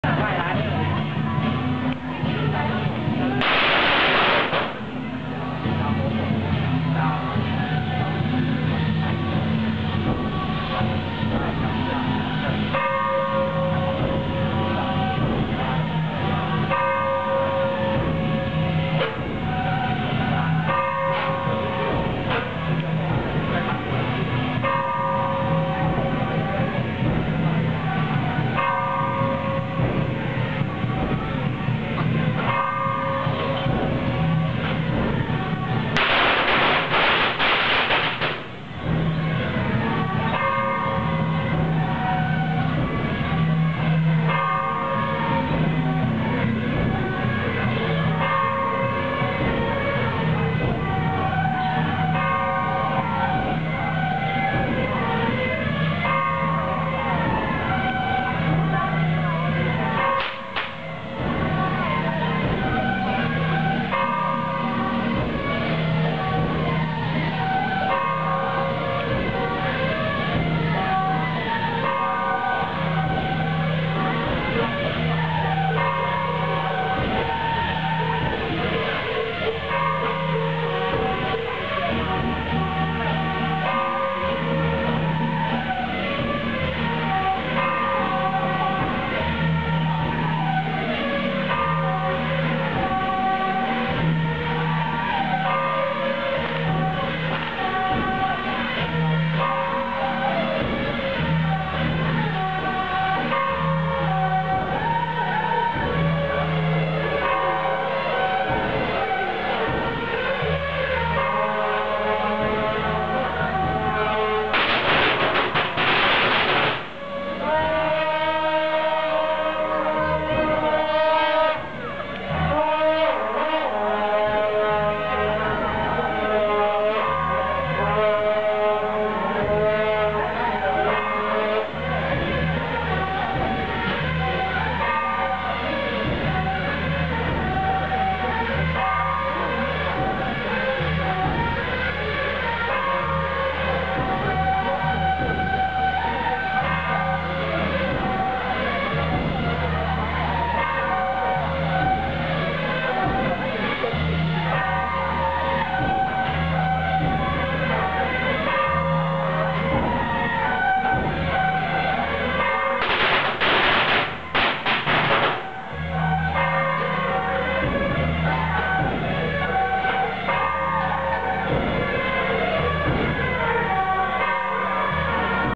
新竹都城隍二公子、謝、范將軍查夜暗訪--關帝廟拜廟
辛卯年晉封威靈公新竹都城隍二公子、謝、范將軍查夜暗訪，關帝廟拜廟